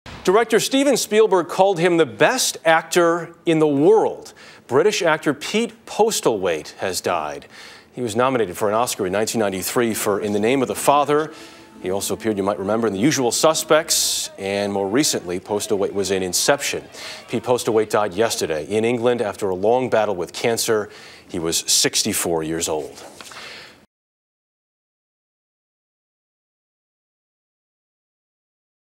Oscar Nominated Actor Pete Postlethwaite Dies
Oscar nominated British actor Pete Postlethwaite died after a long battle with cancer.